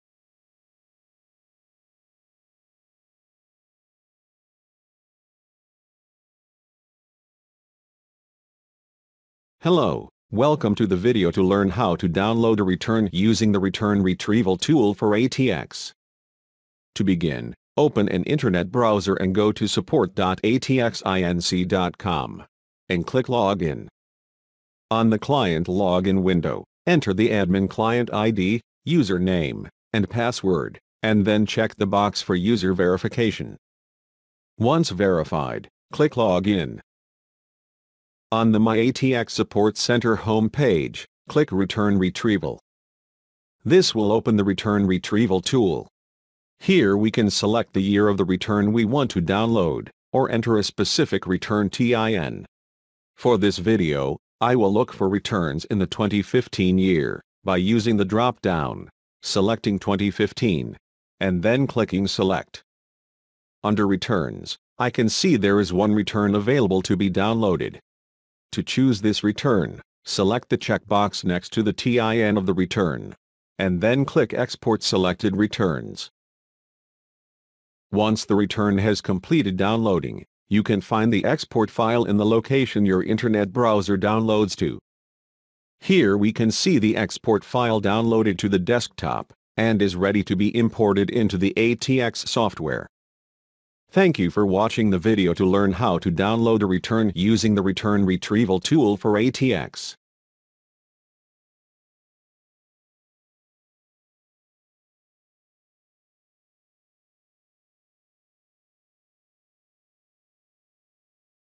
ATX™: Using Return Retrieval to Download a Return
This video explains how to use Return Retrieval on the ATX™ Customer Support website to download return files.